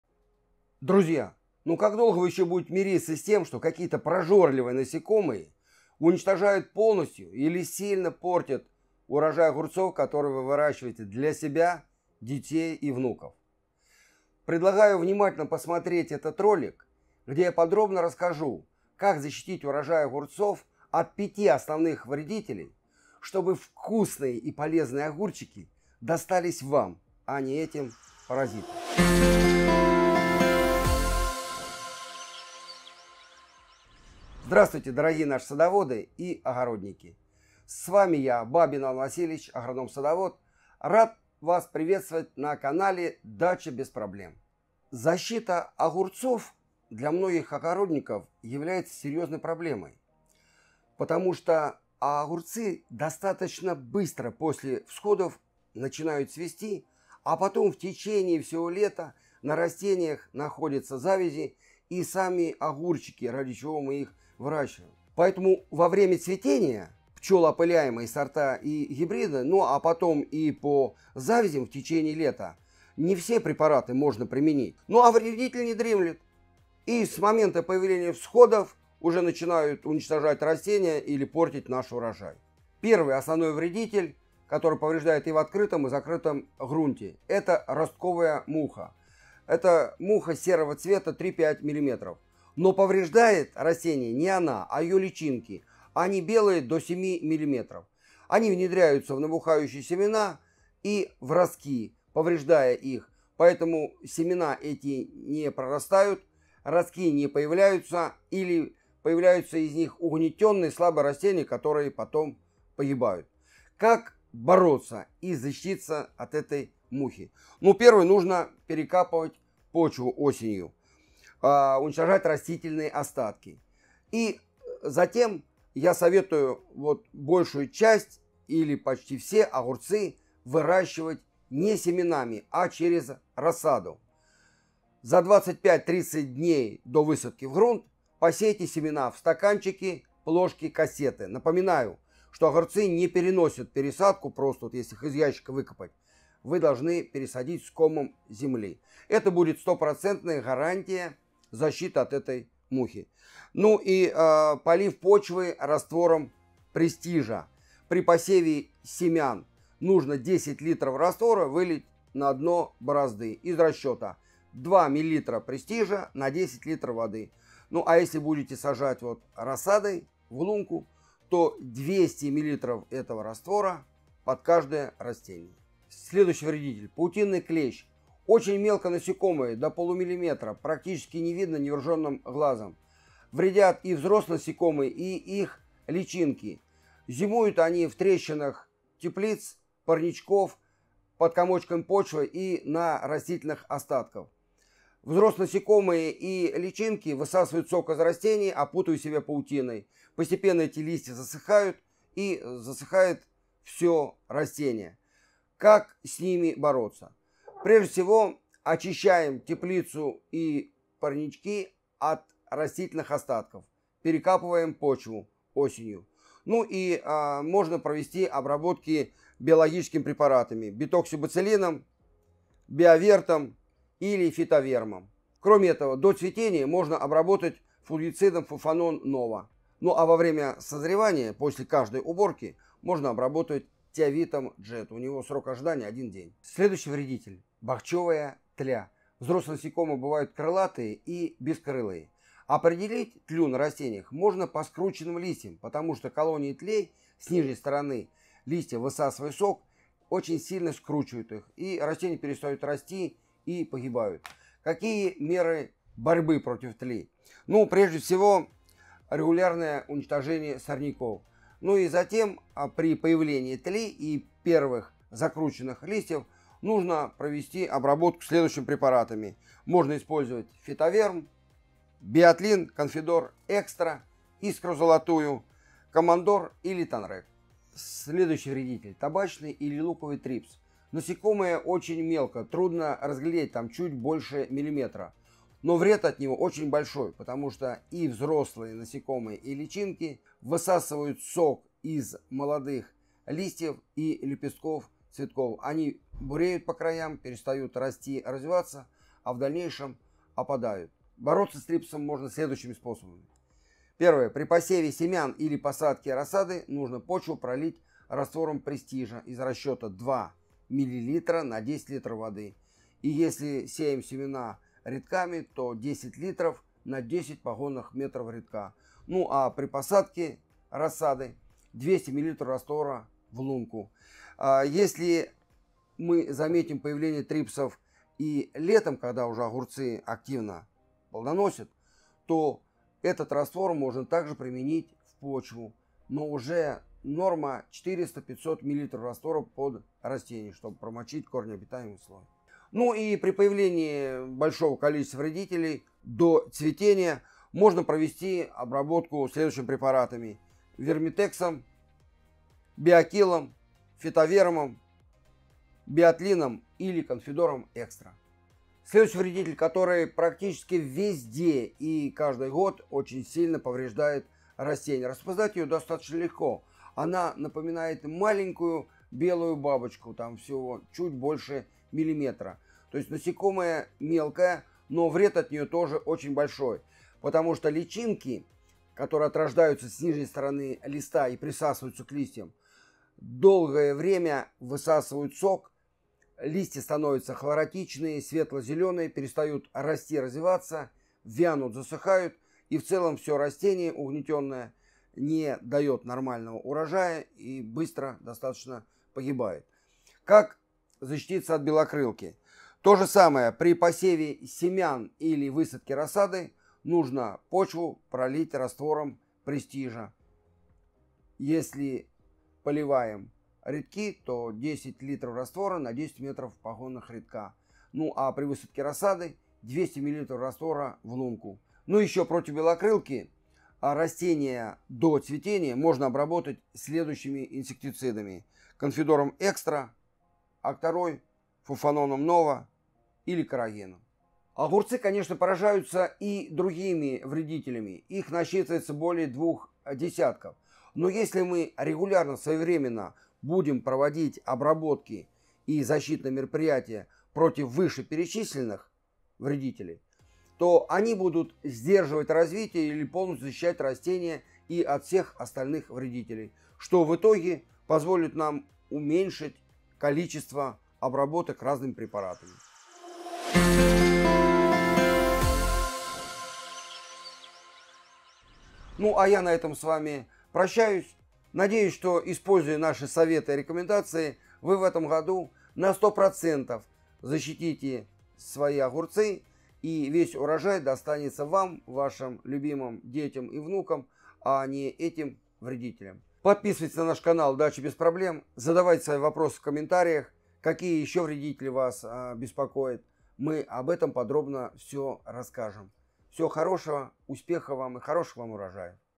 5 главных вредителей огурцов. Эффективные меры борьбы
В сегодняшнем ролике мы поговорим о том, как защитить свой урожай огурцов от безжалостных вредителей. Наш эксперт Иван Васильевич Бабин расскажет, кто является главными вредителями огурцов и какие меры защиты необходимо применять от них. Получите здоровый урожай огурцов вместе с каналом Дача Без Проблем!

Содержание:
00:00 - 5 главных вредителей огурцов
01:22 - Ростковая муха и как с ней бороться
02:56 - Паутинные клещи
03:56 - Бахчевая тля
04:37 - Табачные (луковые) трипсы
05:58 - Белокрылка
07:48 - Заключение